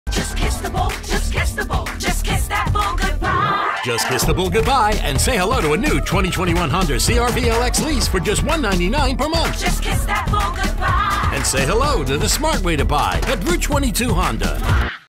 Route 22 Honda CR V BIG Summer Savings
Get over to Route 22 Honda, during their BIG Summer Savings event, where you can save on all your favorite models, including the 2021 Honda CR-V. Whether you're looking to put minimal down or for a low monthly payment, Route 22 Honda offers options for every buyer. Check out all of their BIG Summer Savings offers or get over to their convenient showroom at 75 US 22, Hillside, NJ 07205. Offers expire 7/6/21